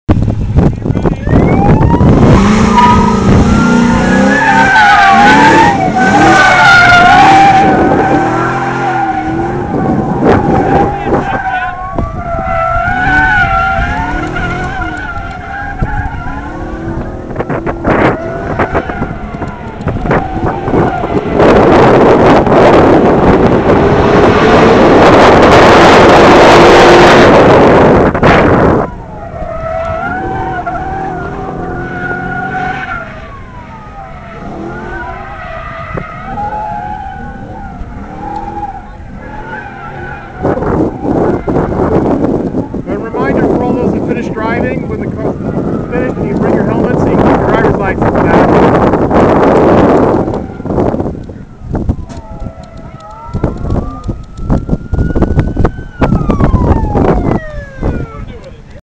Wild ride in a CHP car at Wings and Wheel
This is one of the CHP Demo cars of the Dodge charger. Ride along in the Autocross Course at Wings and Wheels event at the California Aerospace Museum.